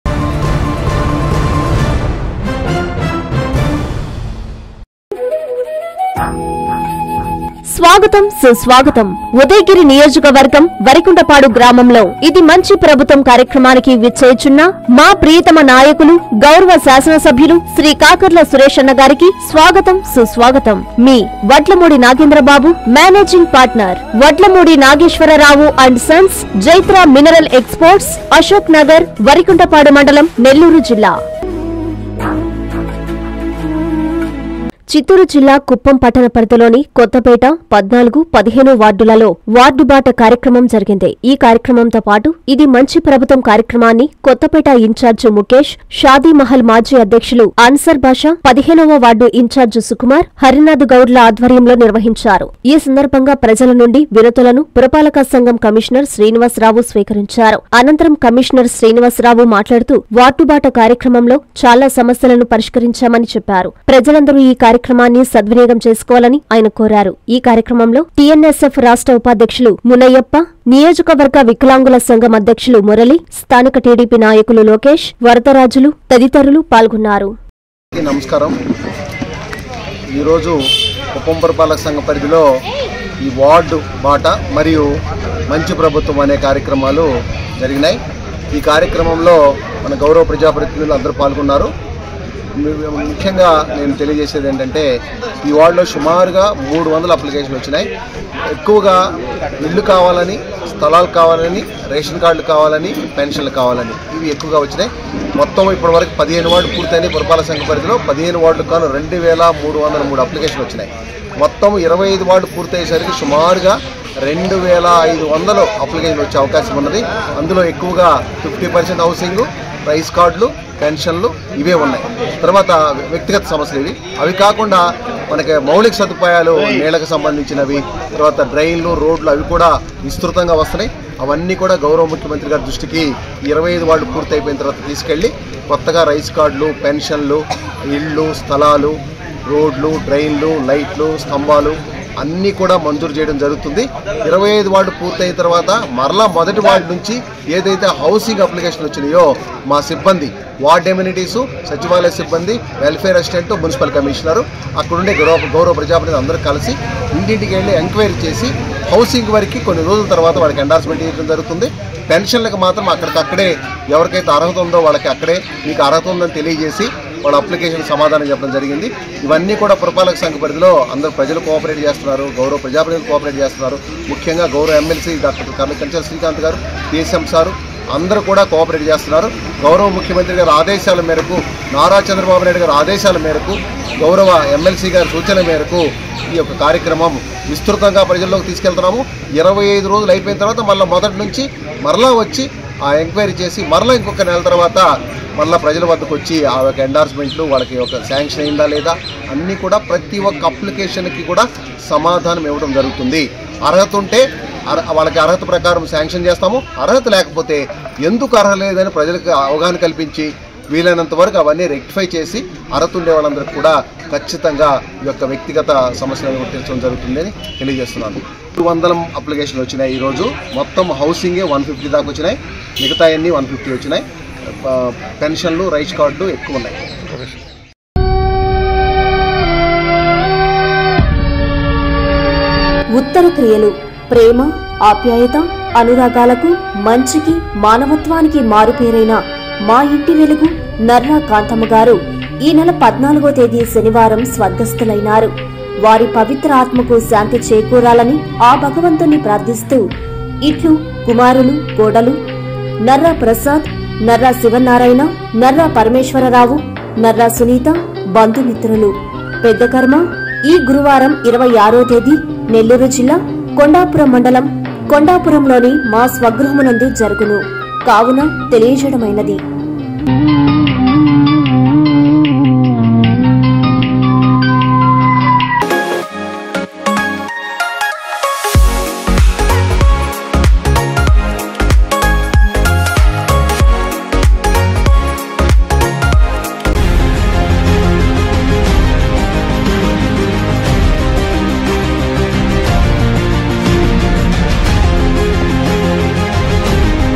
వార్డ్ బాట‌లో స‌మ‌స్య‌ల‌కు ప‌రిష్కారం
- మున్సిప‌ల్ క‌మిష‌న‌ర్ శ్రీ‌నివాస‌రావు
- కుప్పం కొత్త‌పేట 14,15 వార్డుల్లో వార్డు బాట కార్య‌క్ర‌మం